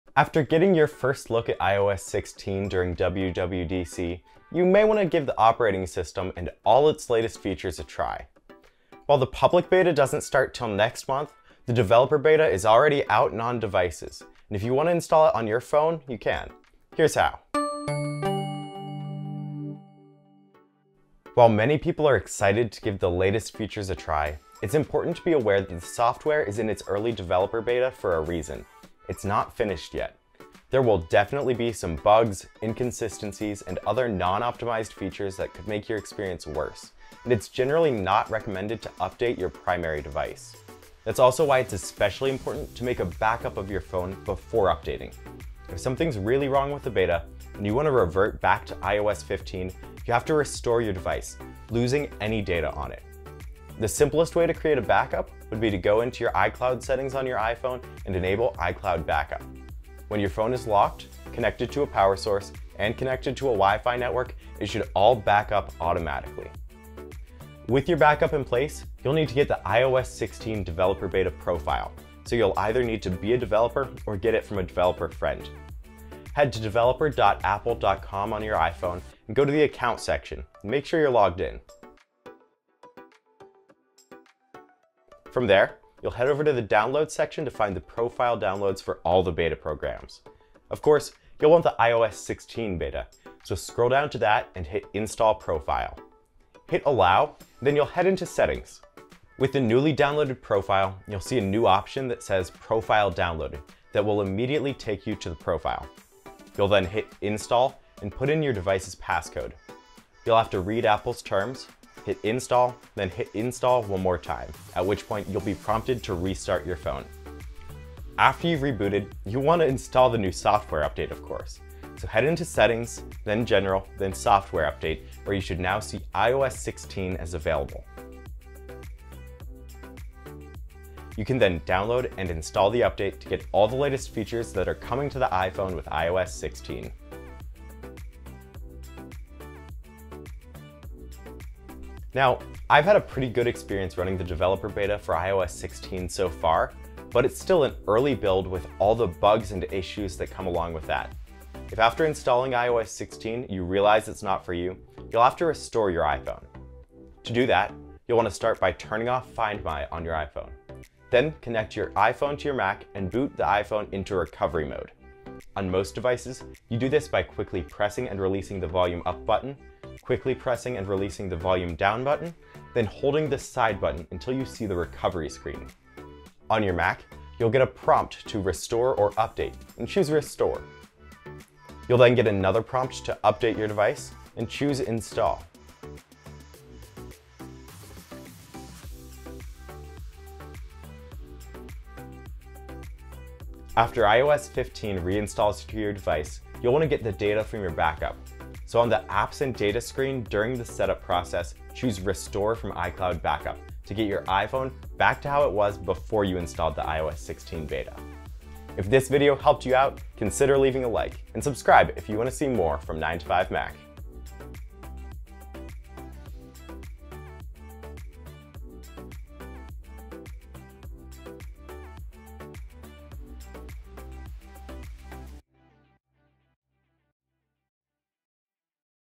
How to install the iOS 16 beta and unlock new features on your iPhone
The iOS 16 developer beta is out and on devices. Here's how to install it on your iPhone (and revert back to iOS 15 if you change your mind).

Comment: 📝 Let me know what you think...

-Sigma 18-35 F1.8
-Manfrotto tripod
-Sigma 150-600 F5-6.3

CHAPTERS:
0:00 Introduction
0:20 Make a backup
1:08 Install iOS 16 beta
2:20 Revert to iOS 15
3:28 Conclusion